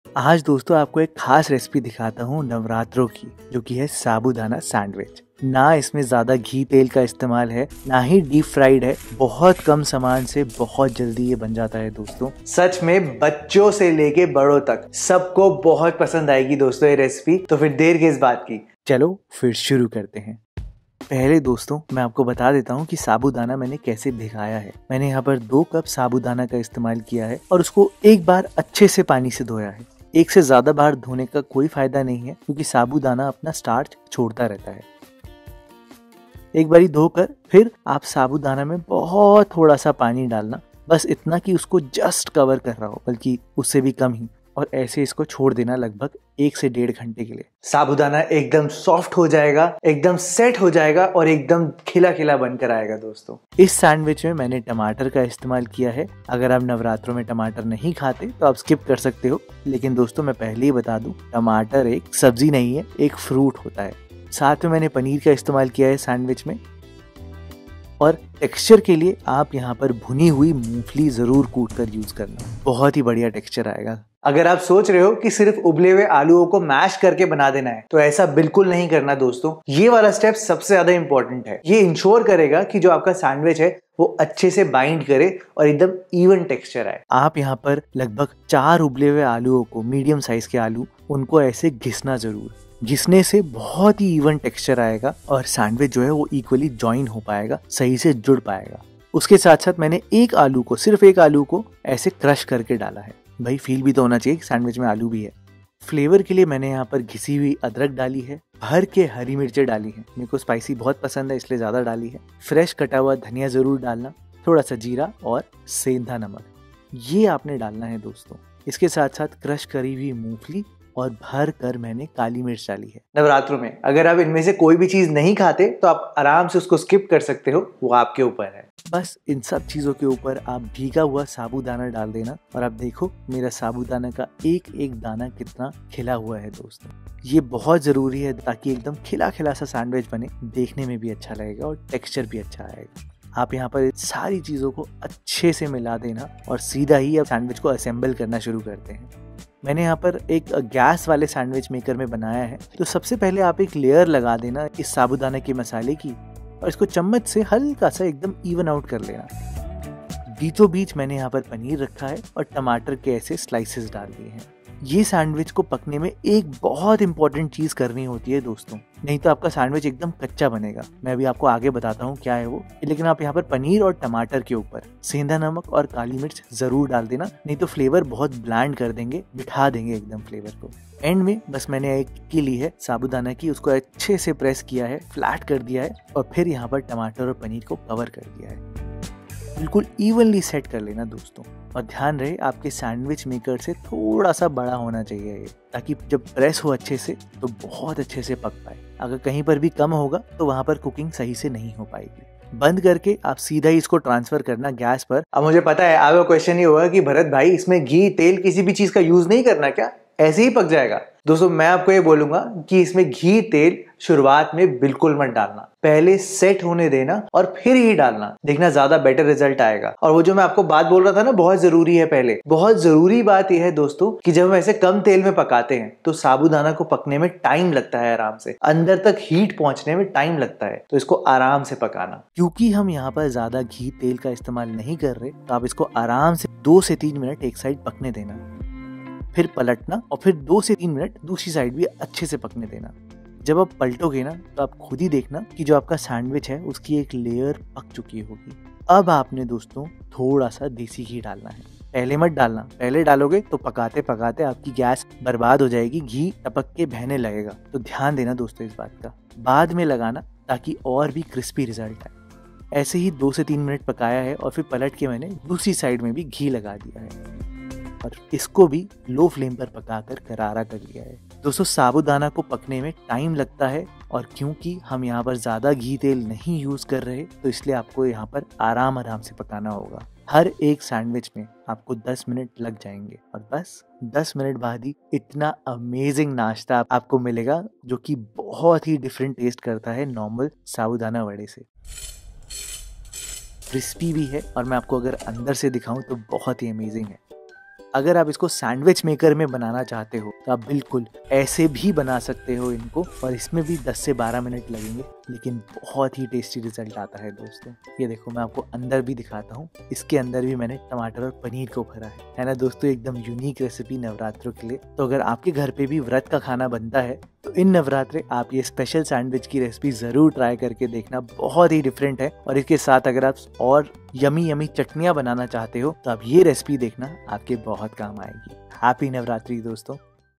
बिना तले साबूदाना की ऐसी मज़ेदार नई रेसिपी एकबार बना लिए तो बिना व्रत भी रोज बनायेंगे- Vrat Sandwich
2 Cups Sabudana
5 Boiled Potatoes
1 tsp Seendha Namak
1 tsp Black Pepperpowder
2 - 3 Green Chillis
Coriander Leaves
1 tsp Jeera
1.5 Inch Ginger
1/4 Cup Roasted Peanuts